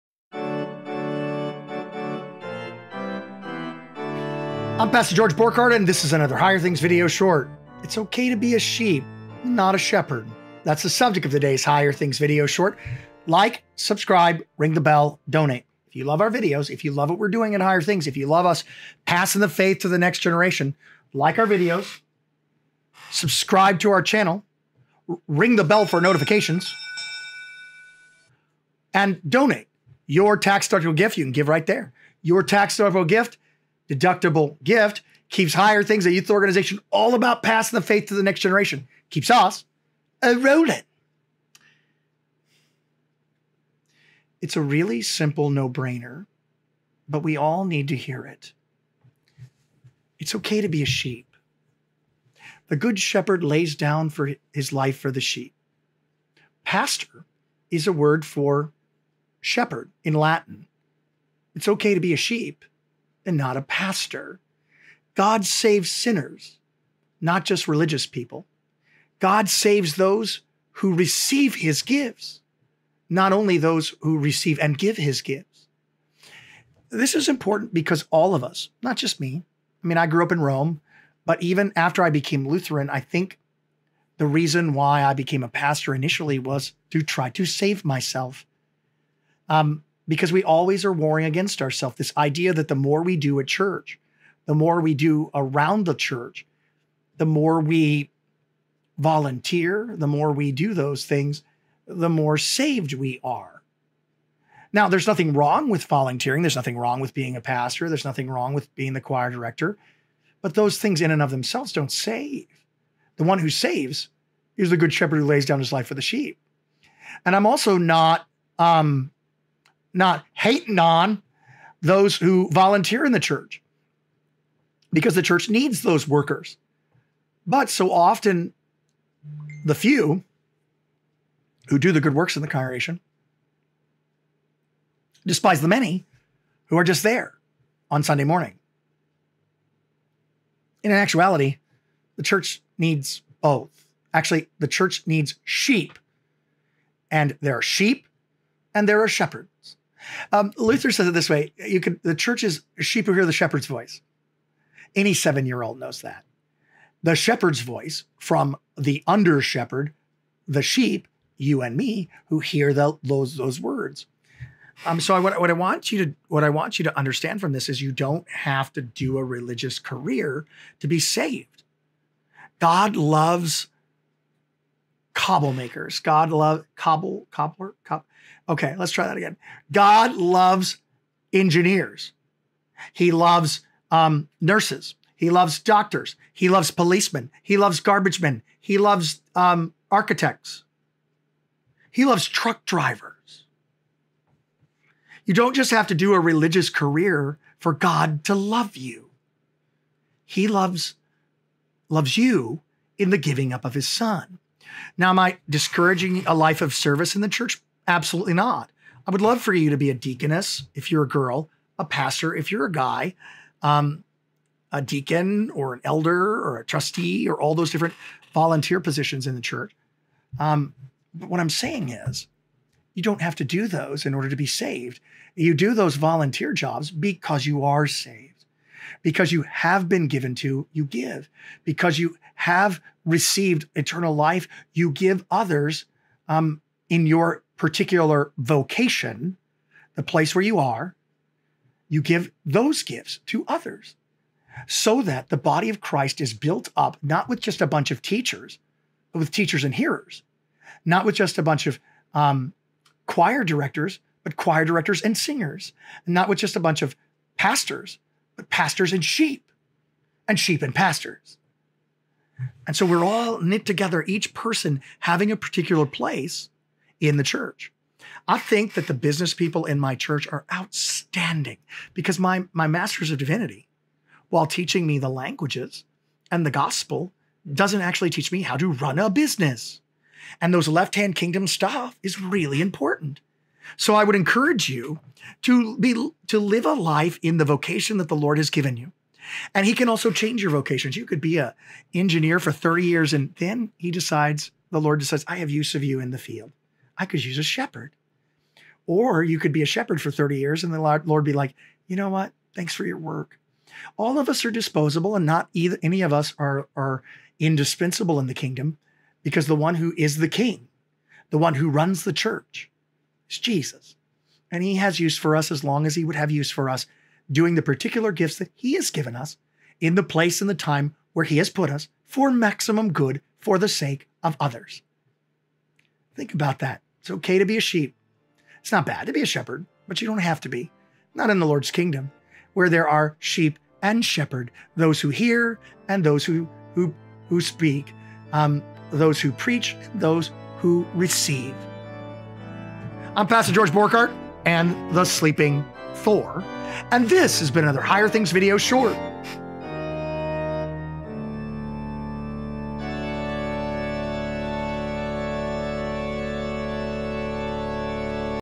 0429 It's OK to Not be a Pastor - A Higher Things® Video Short
It's ok to be a sheep. God saves sinners, not just religious people. It's easy to start thinking that the more we do at church, with church, around church...the more saved we are. And then we start to despise the ones who only show up on Sundays. There's nothing wrong with serving the church, it's a good thing to do. But it doesn't save you. Only Jesus saves you.